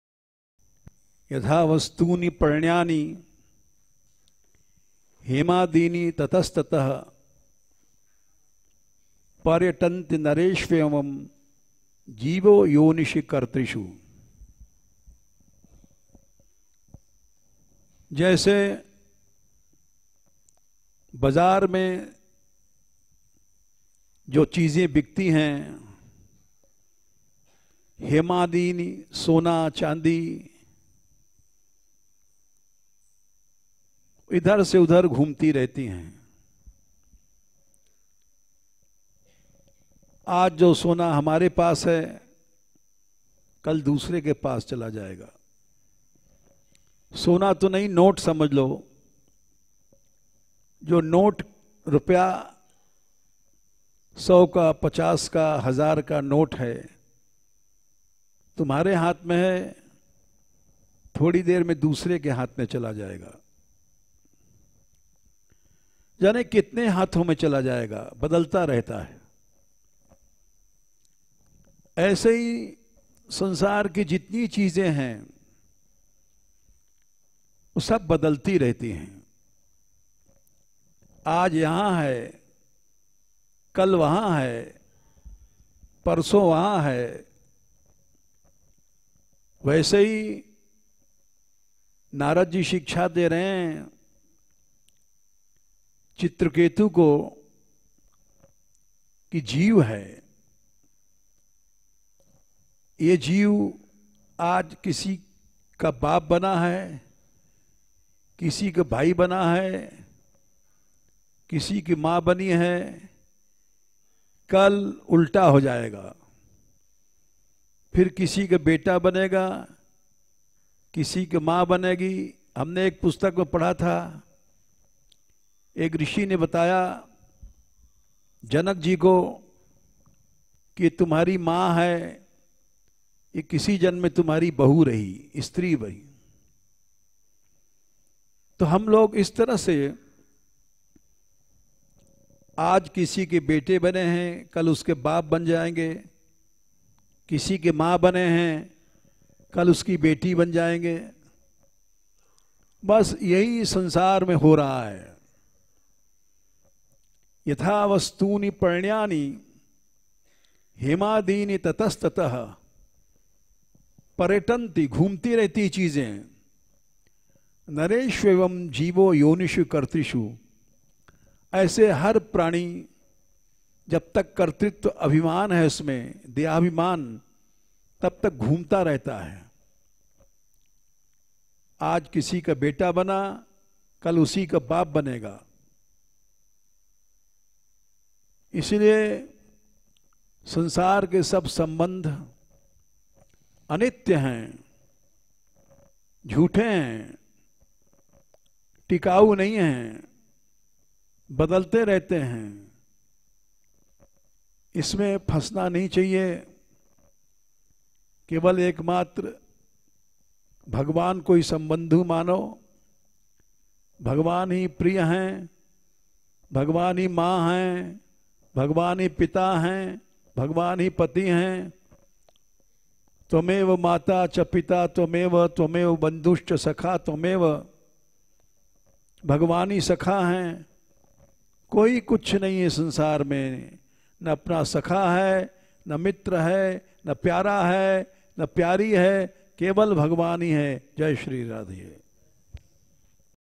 after nb-2013-01-09-Sansar ki nashvarata(Shri Ramesh baba ji Maharaaj Maan mandir Barasana)
Live Satsang from maan Mandir Barasana by Shri Ramesh baba Ji Maharaj
After Night Sankeeratan-(ans)-Sasar ki nashvarta
Dated:Jan 09,2013